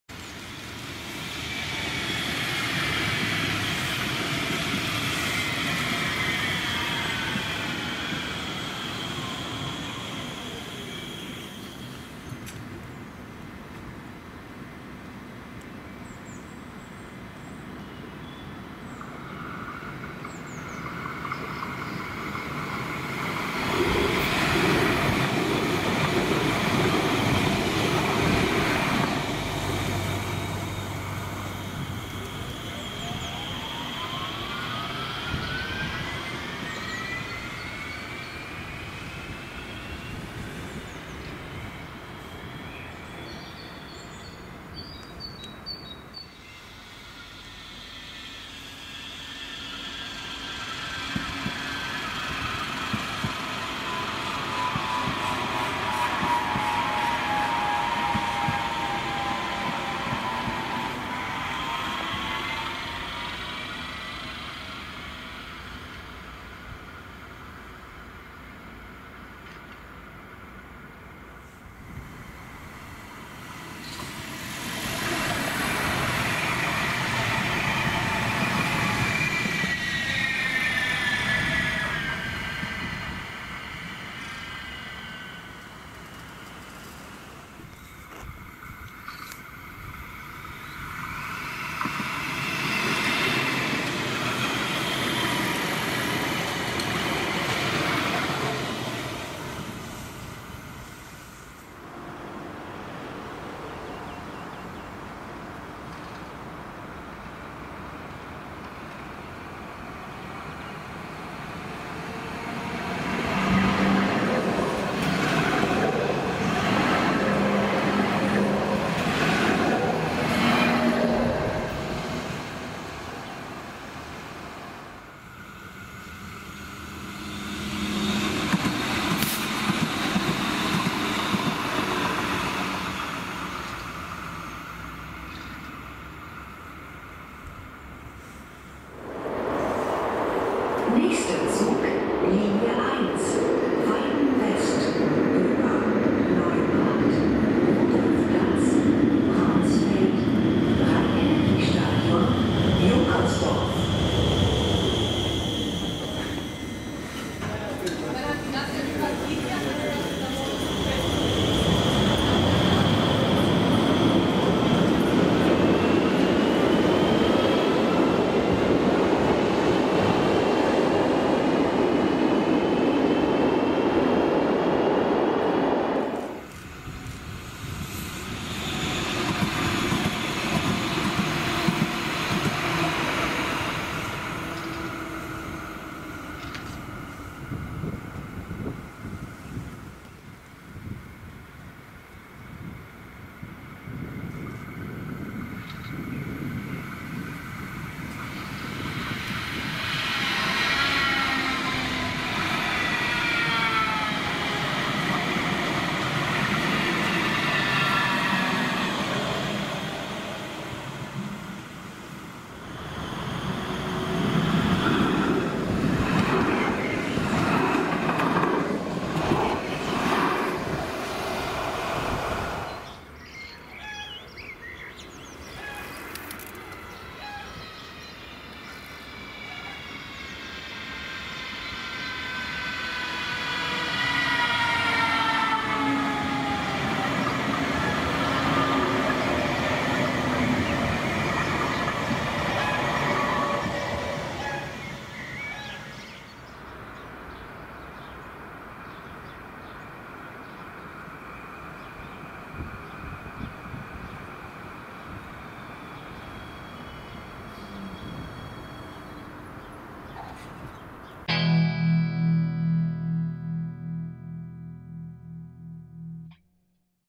Köln Stadtbahn [Trams in Cologne]
You can check the trams (Stadtbahn) in Köln (Cologne).
It consists of 11 lines, operated by KVB (Kölner Verkehrs-Betriebe) with low and high floor trains.

Train types.
K5000, K4500, K4000, B80S.